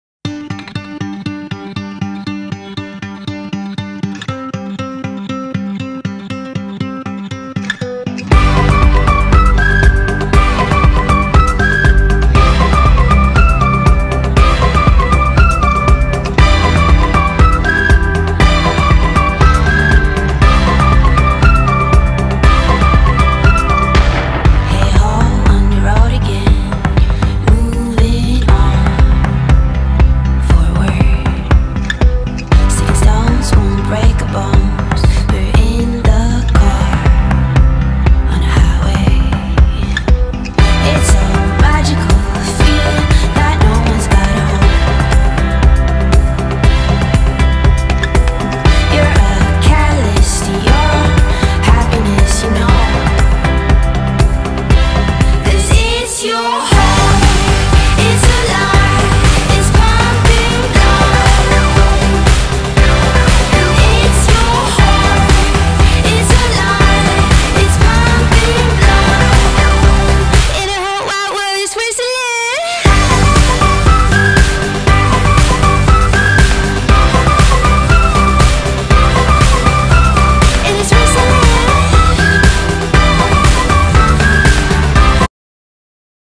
Alma|| March- April
Alma has been going amazingly recently and we didn't go to Eden Valley as I felt we weren't ready for it, going to a PC ODE on 3rd May and then hopefully Hutton in the Forest as her first one  Please sub:)     Neve and Alma xoxo